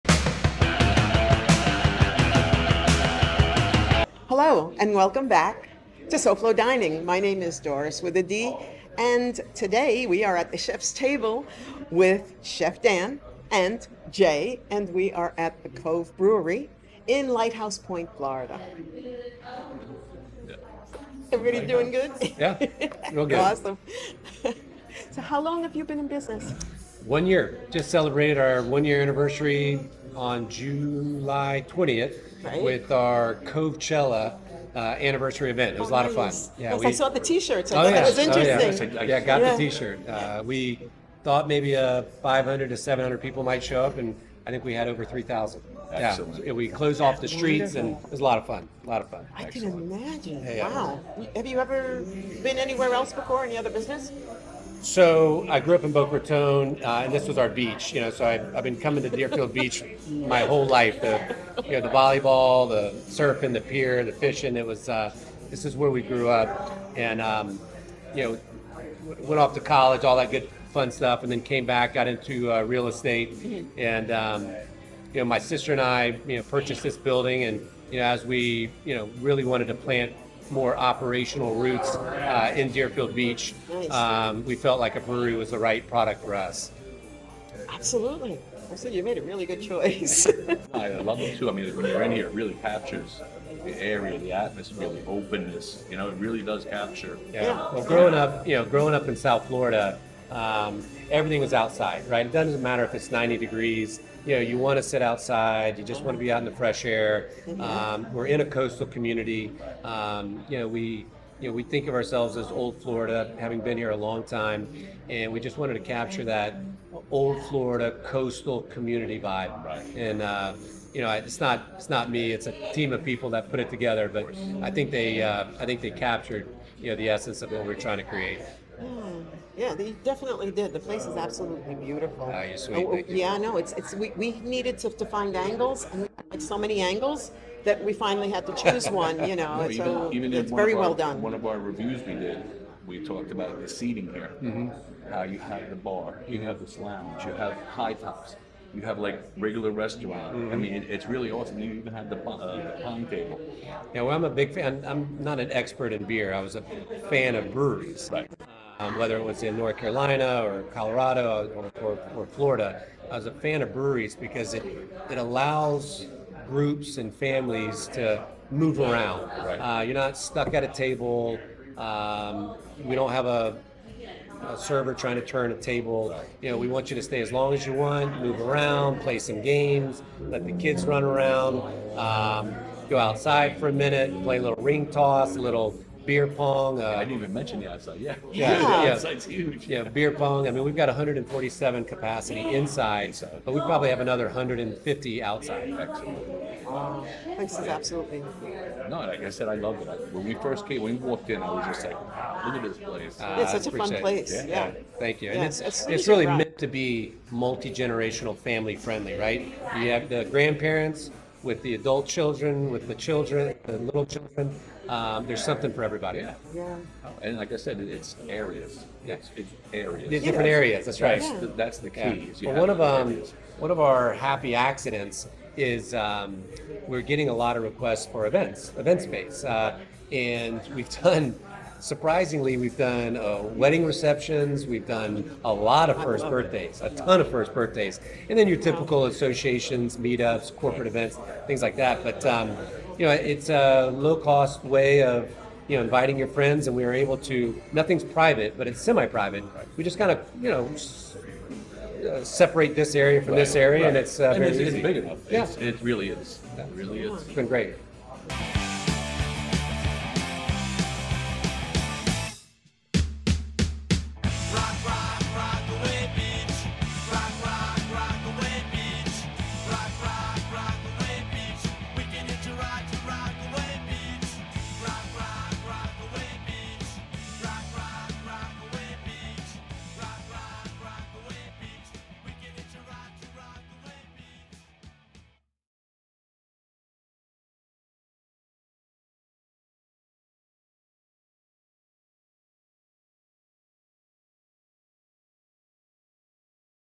Discover Your New Favorite Beer at The Cove Part 1
Don't miss out, fellow SoFlo foodies! This video will have you craving a cold one from a new spot.

Want to know the inside scoop on the hottest new brewery in Broward County?  Check out this video I found on the SoFloDining YouTube channel.  It's an interview with the owners of The Cove Brewery, located in Deerfield Beach.

They talk about their inspiration for creating a community space where people can gather over delicious food and beers.  And speaking of beer, they offer a wide selection of IPAs, stouts, and more, with something to please every palate.

If you're looking for a new place to try,  you won't want to miss this one!

P.S. This video is the full video that is only available on the SoFloDining Page and SoFloDining YouTube channel. You'll have to watch it to see everything they have to offer!